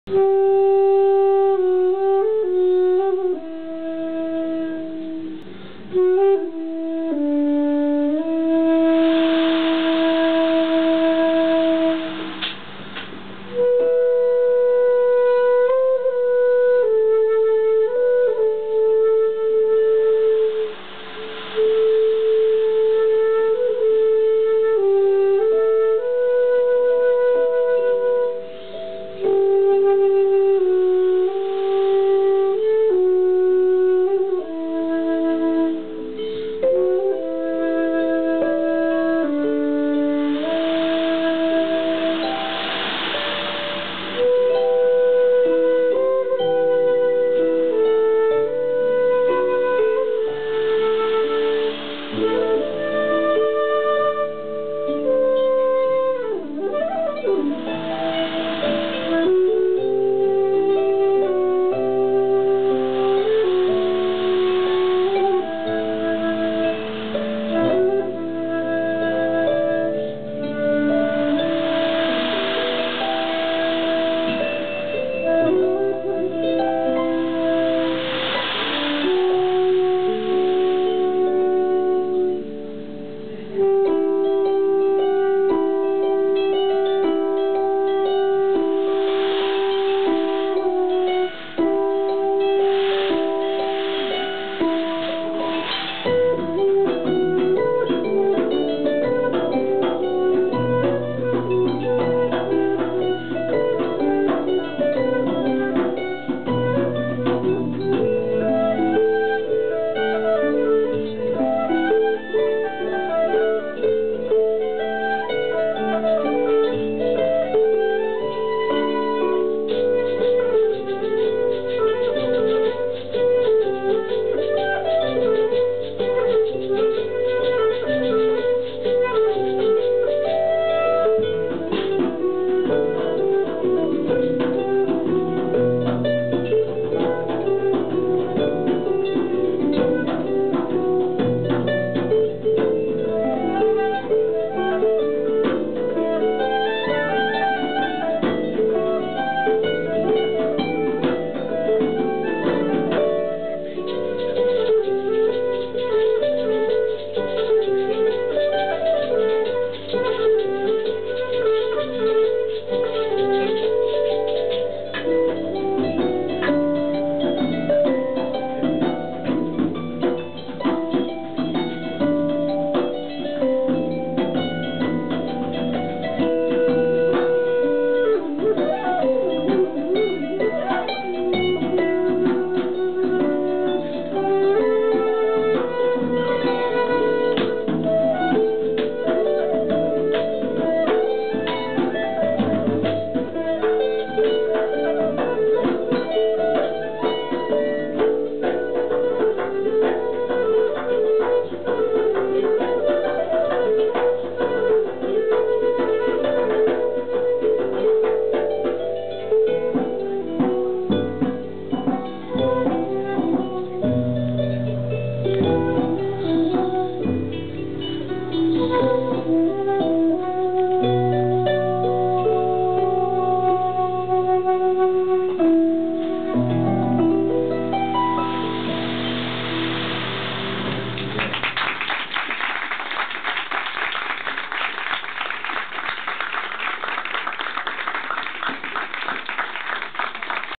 Giovanni Block e Gianluca Rovinello. Concerto flauto e arpa al kestè part II
Dopo lo scioglimento del gruppo di musica celtica Eala nel lontano 2005, larpista Gianluca Rovinello, (leader dellorchestra darpe di fama Nazionale "An arperc" e arpista ufficiale della "Dioniso FOlk Band" ) e Giovanni Block (vincitore di premi come il Tenco, Musicultura, De andrè e segnalato dalla Critica e da Cantautori del calibro di Sergio Cammariere e Morgan tra i migliori cantautori della scena emergente italiana.) si incontrano nuovamente per un viaggio che ripropone le più celebri arie della tradizione celtica, attraverso i suoi più antichi e nobili strumenti.

Dalle origini della musica irlandese fino al barocco di O' Carolan, il flauto Block e l'arpa Rovinello si fondono in una affascinante e suggestiva performance artistica.